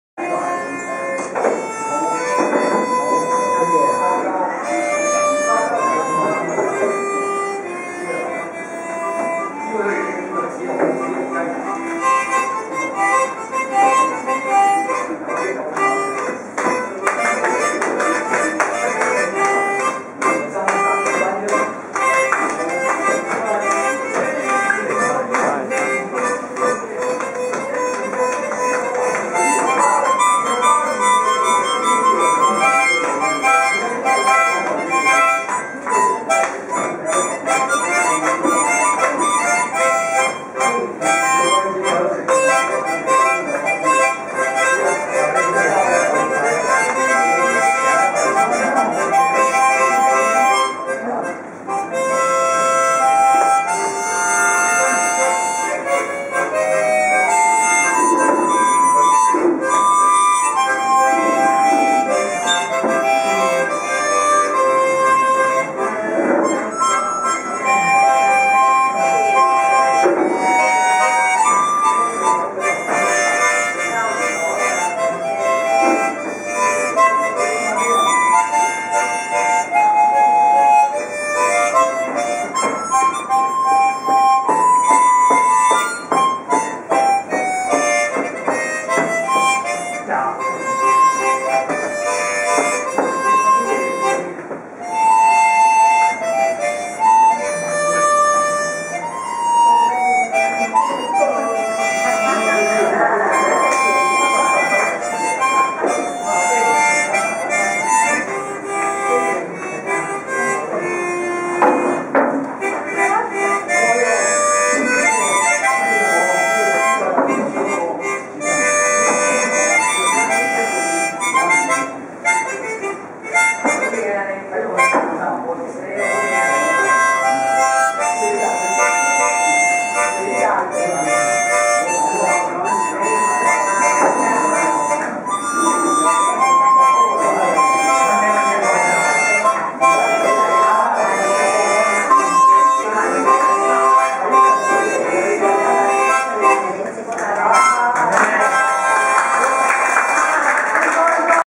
開心口琴隊
從中式建築看中國文化
2011年6月29日《屏山文物徑導賞》
導賞講師：蘇萬興先生
參與團體：開心口琴隊